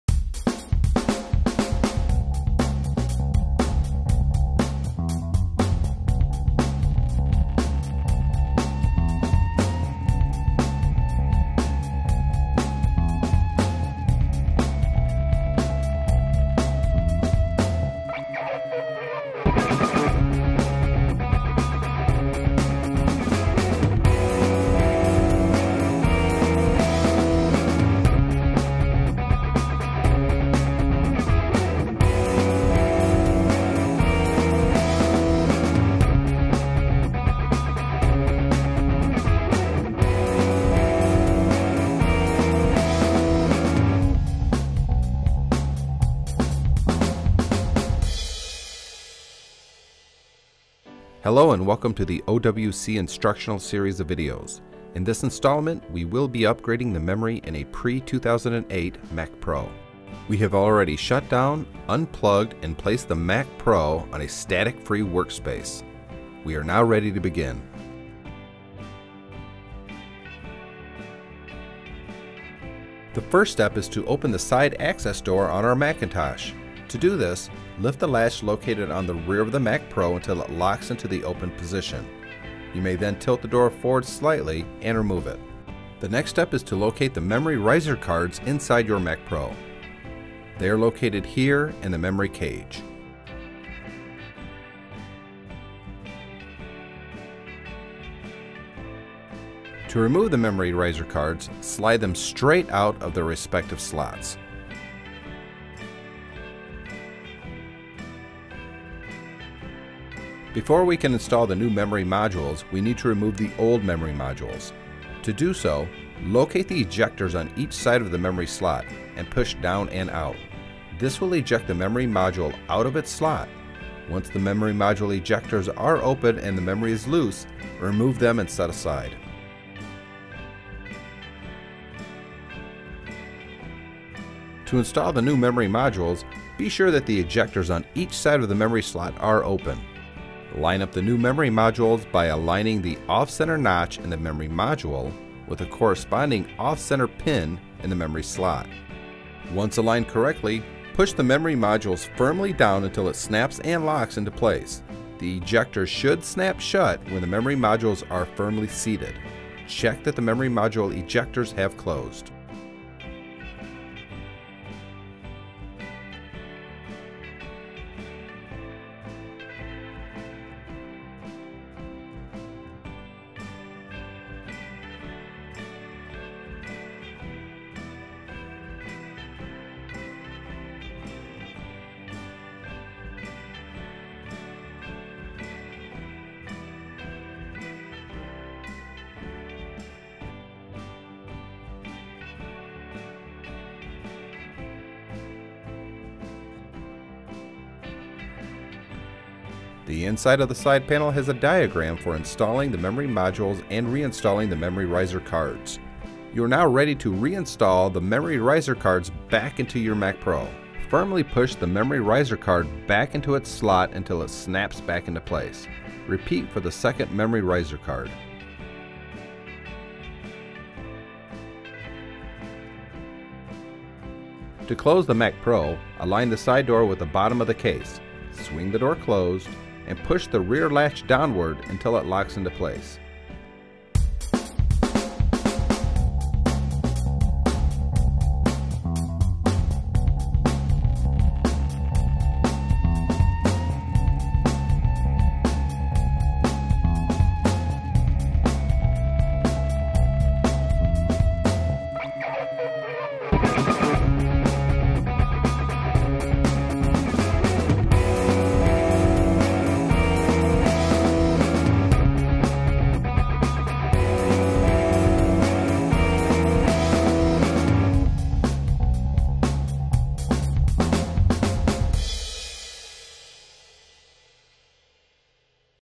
Mac Pro (Pre-2008) Memory Installation Video
OWC's step-by-step video of how to install memory in an Apple Mac Pro (Pre-2008). Model ID: MacPro1,1 or MacPro2,1.